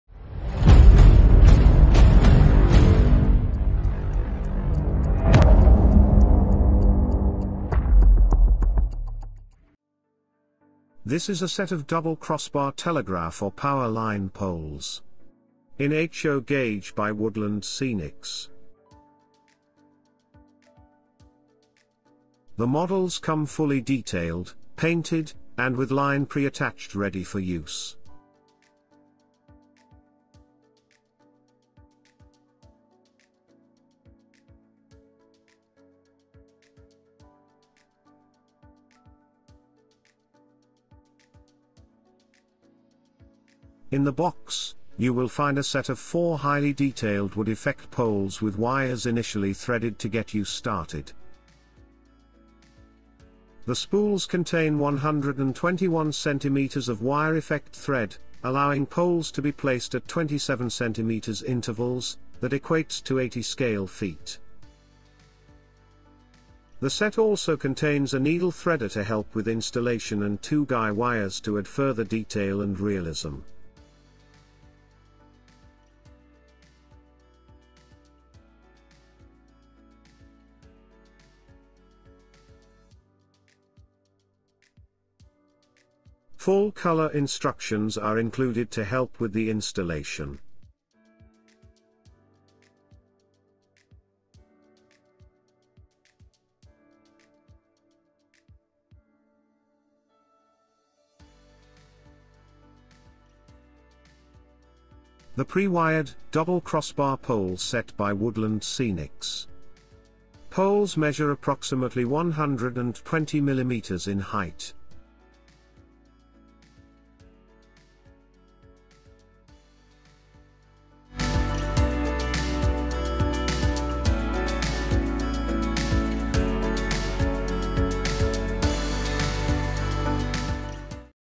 341 - Woodland Scenics US2266 Pre-Wired Double Crossbar Poles HO gauge
Unboxing and presentation of a set of double crossbar telegraph or power line poles in HO gauge by Woodland Scenics.
The models come fully detailed, painted, and with line pre-attached ready for use.

In the box, you will find a set of four highly detailed wood effect poles with wires initially threaded to get you started.
The spools contain 121cm of wire effect thread, allowing poles to be placed at 27cm intervals, that equates to 80 scale feet.
The set also contains a needle threader to help with installation and two guy wires to add further detail and realism.
Full colour instructions are included to help with the installation.